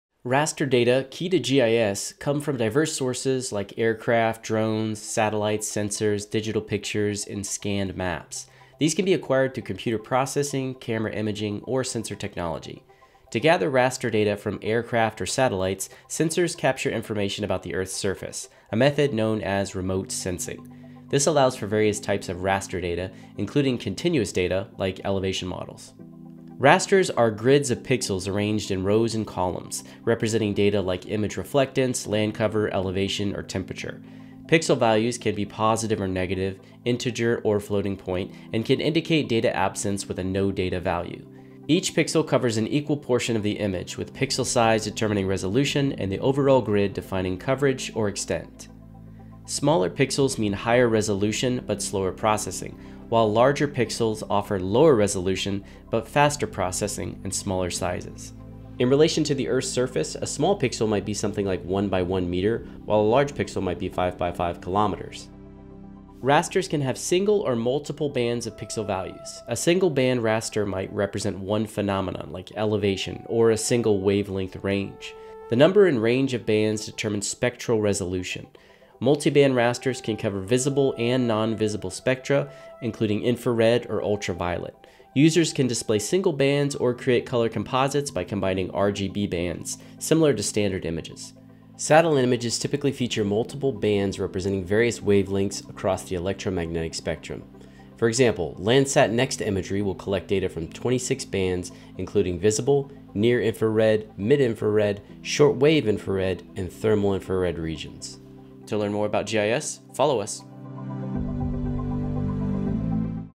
What is Raster Data in GIS? | Raster vs Vector Graphics Explained
Understand what raster data is and how it powers GIS through remote sensing from satellites and drones, with pixel resolution and bands revealing Earth’s surface details.

Landsat Visuals courtesy of NASA's Scientific Visualization Studio